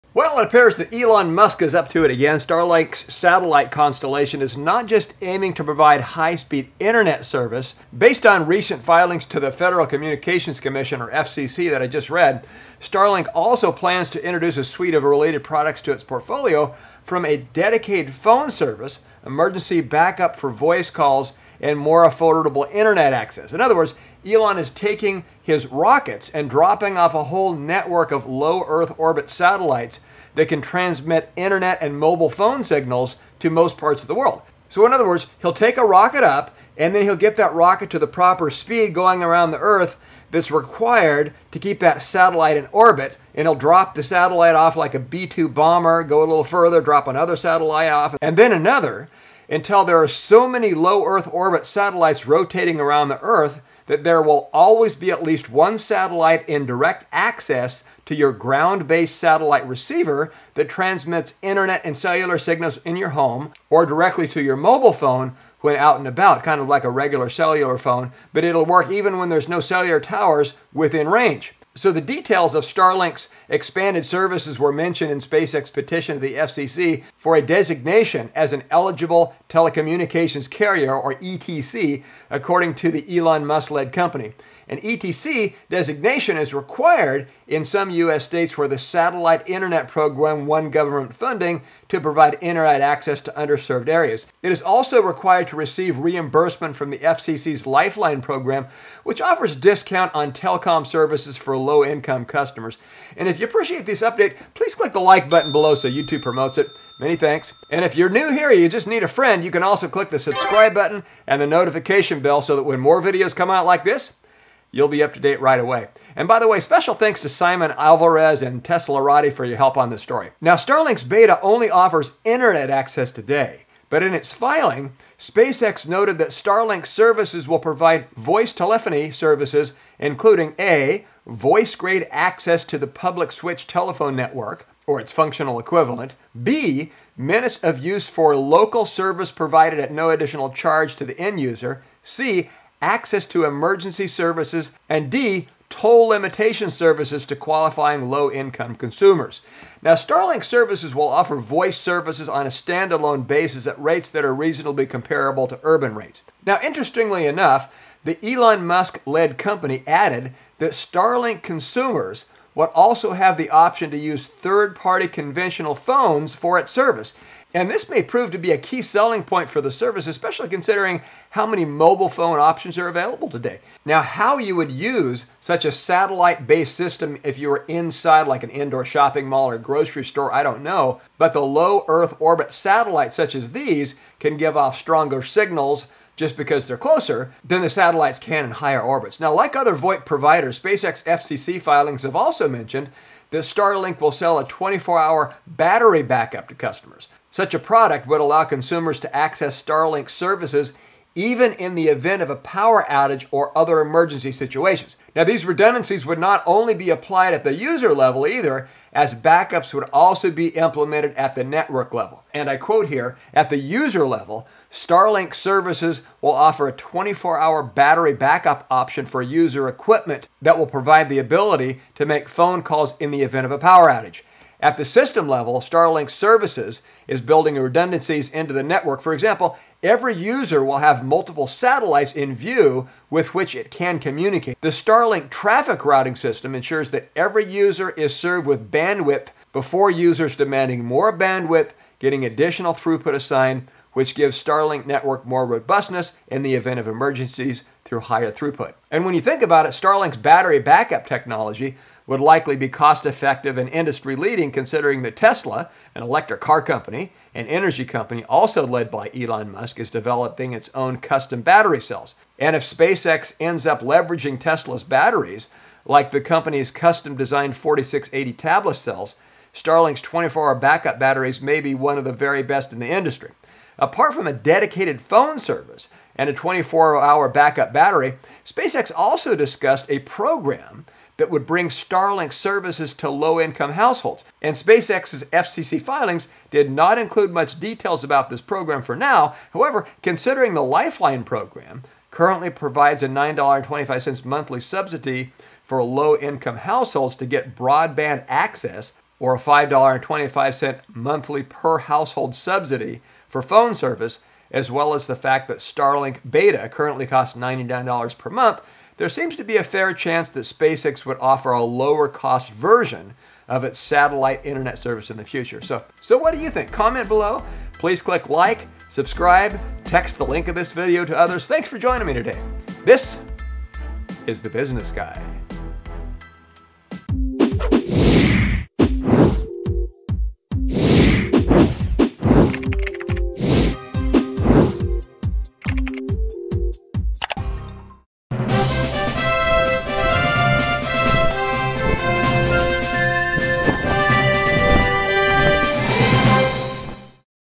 Elon Musk's Starlink Mobile Phone Service (AKA Tesla Phone)
Details about the Starlink Network mobile phone (cellular phone) service, how it works and how much it costs.